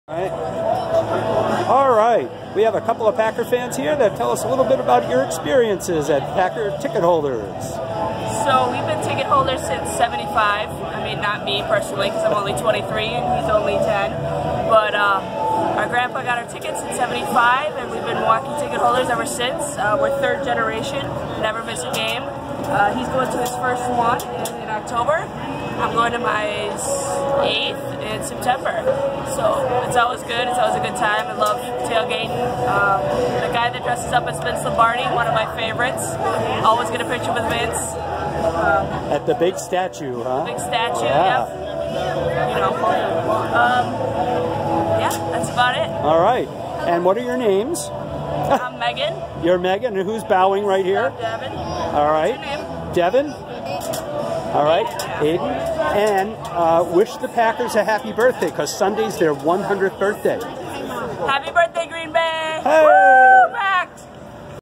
Fun at the Fair!
Here are two Packer fans at Wisconsin's State Fair looking forward to the football season!